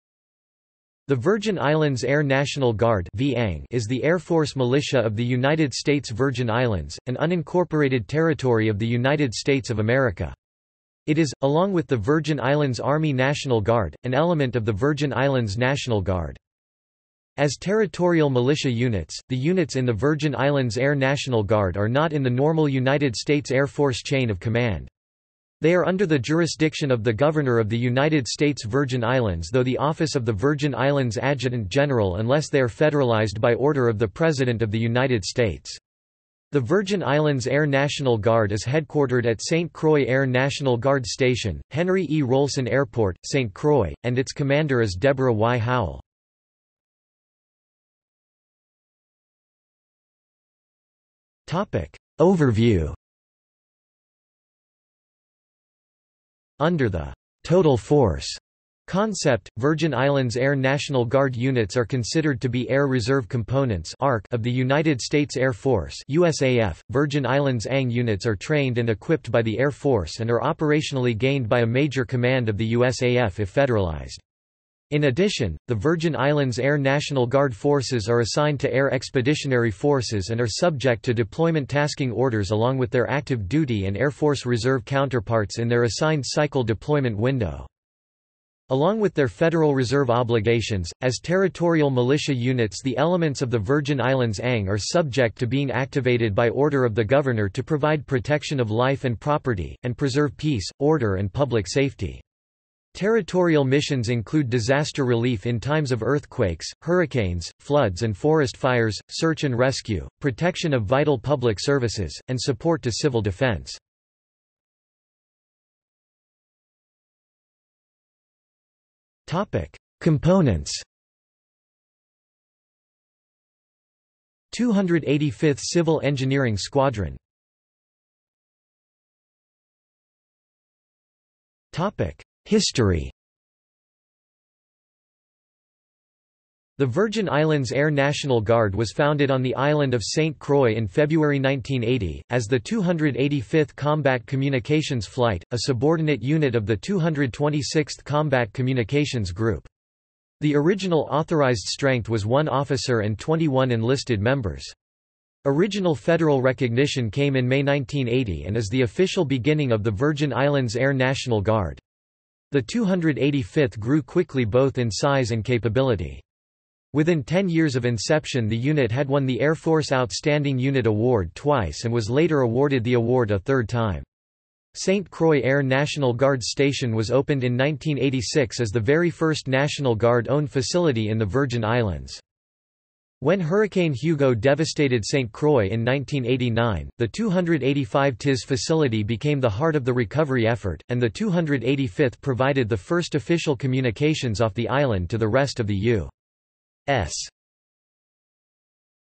Virgin Islands Air National Guard | Wikipedia audio article
This is an audio version of the Wikipedia Article:
Virgin Islands Air National Guard

00:00:47 1 Overview
00:01:55 2 Components
00:02:08 3 History

- Socrates

SUMMARY
The Virgin Islands Air National Guard (VI ANG) is the air force militia of the United States Virgin Islands, an Unincorporated Territory of the United States of America. It is, along with the Virgin Islands Army National Guard, an element of the Virgin Islands National Guard.
As territorial militia units, the units in the Virgin Islands Air National Guard are not in the normal United States Air Force chain of command. They are under the jurisdiction of the Governor of the United States Virgin Islands though the office of the Virgin Islands Adjutant General unless they are federalized by order of the President of the United States.  The Virgin Islands Air National Guard is headquartered at St Croix Air National Guard Station, Henry E. Rohlsen Airport, St. Croix, and its commander is Deborah Y. Howell.